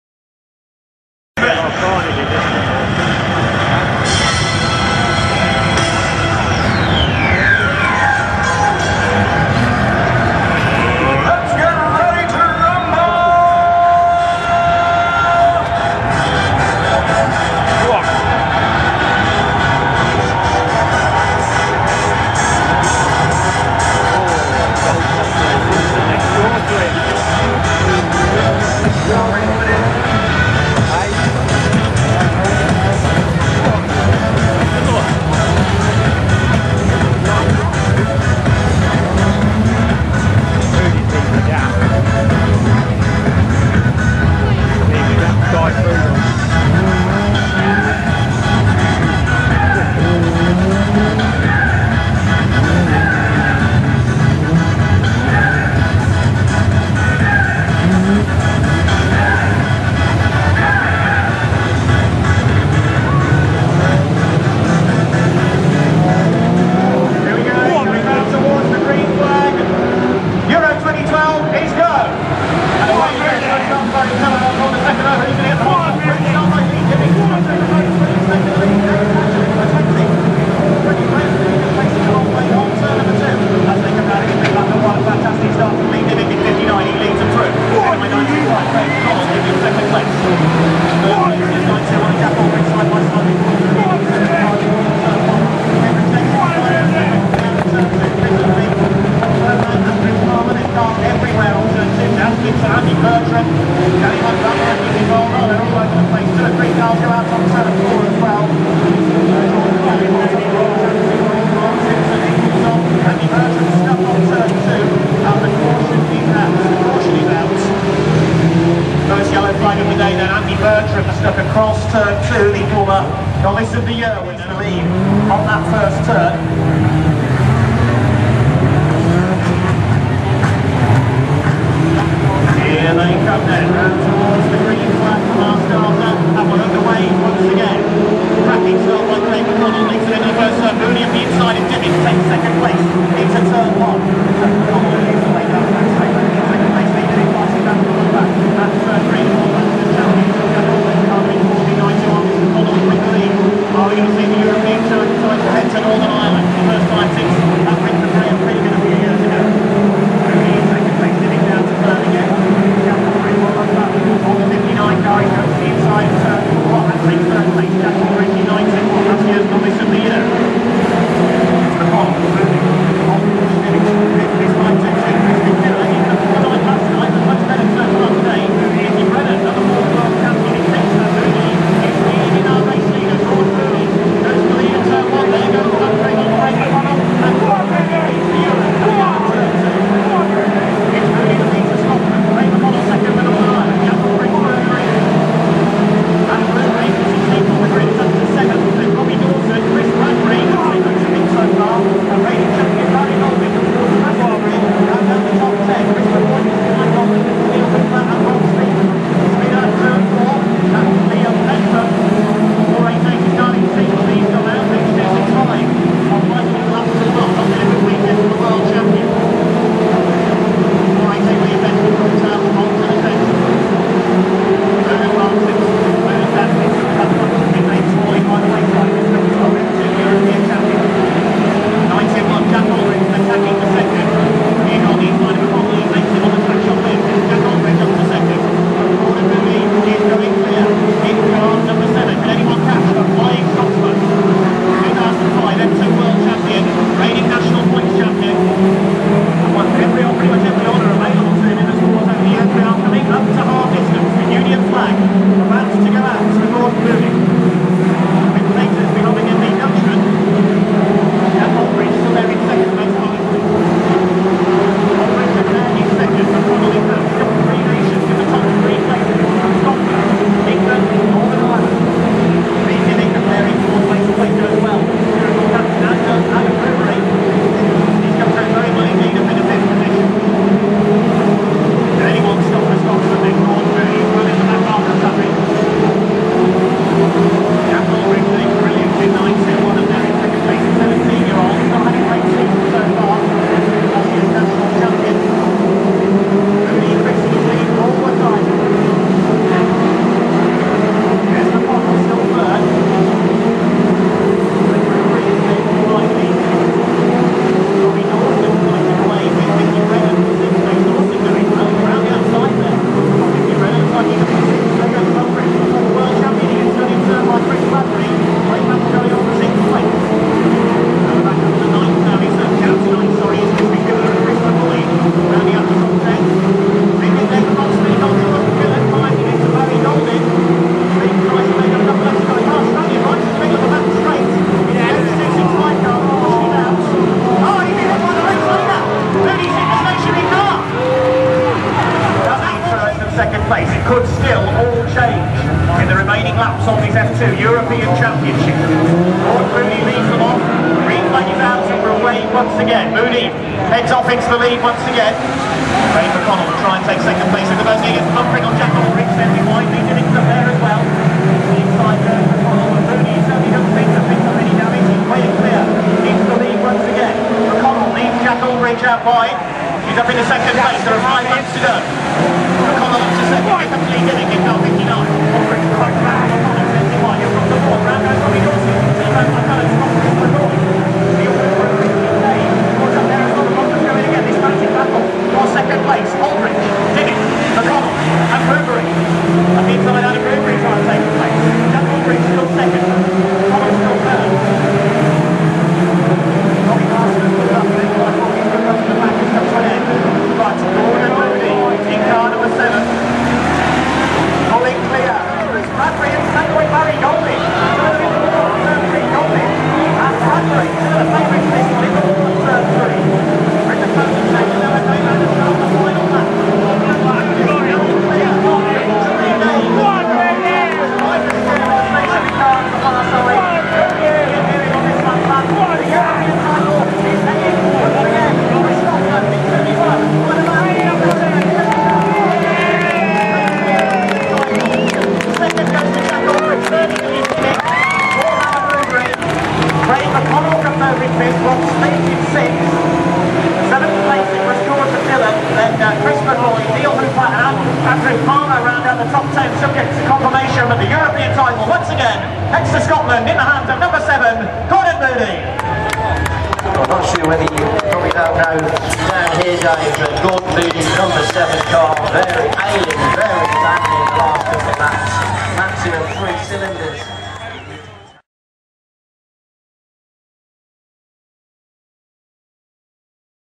Brisca F2 European Championship 2012 Gordon Moodie Aldridge Dimmick Goldin Speak Bradbury
F2 European Championship 2012 from Northampton International Raceway. Not what I usually film, but as its a major event for the 2's, I thought it might produce a good race.